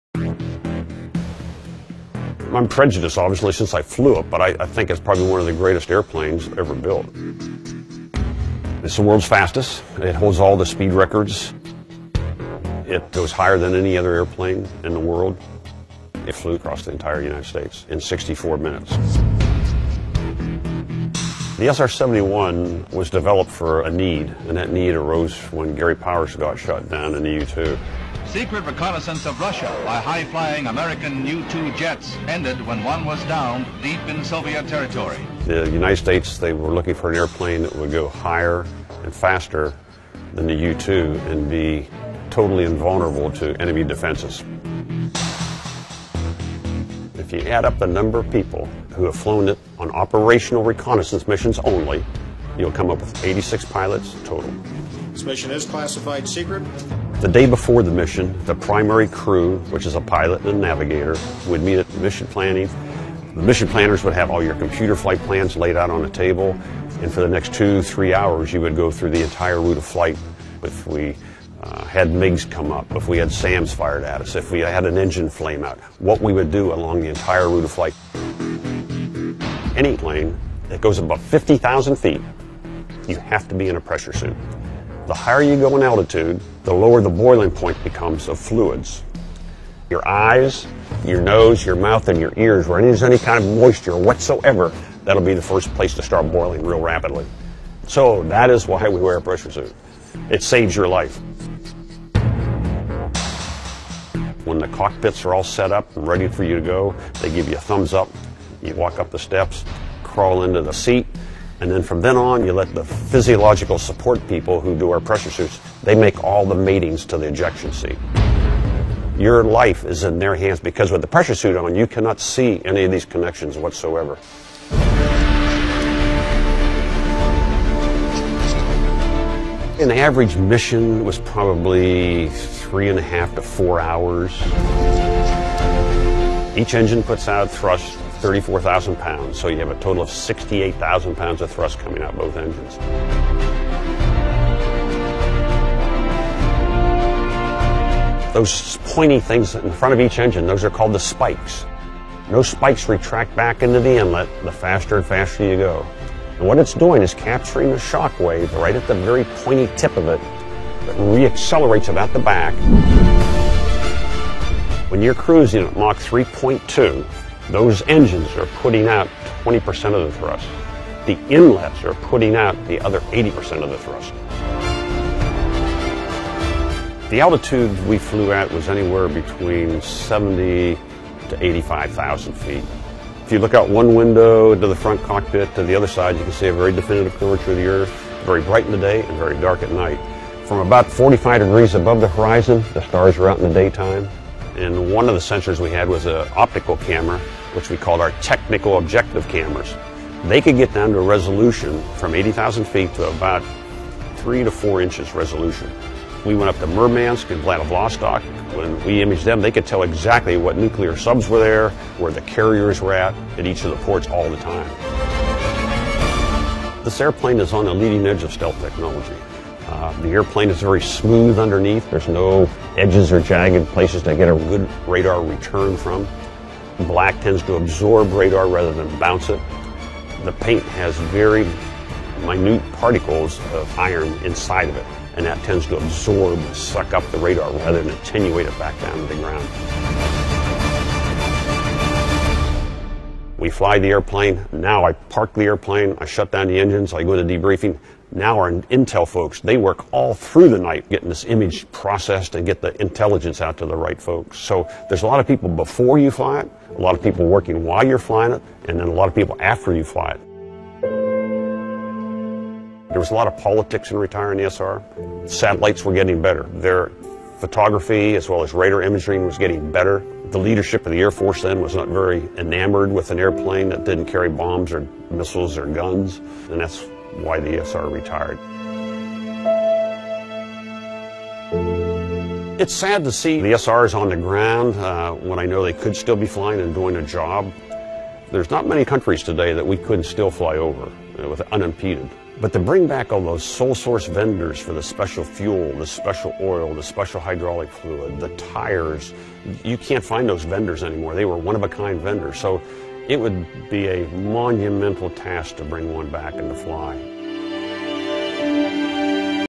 SR 71 Blackbird
It was a plane which flew at the edge of space; so high that most other jet engines would seize because of the lack of air. A plane that flew so fast that its airframe heated and grew during flight. A plane that, if needed, could outrun missiles launched to bring it down.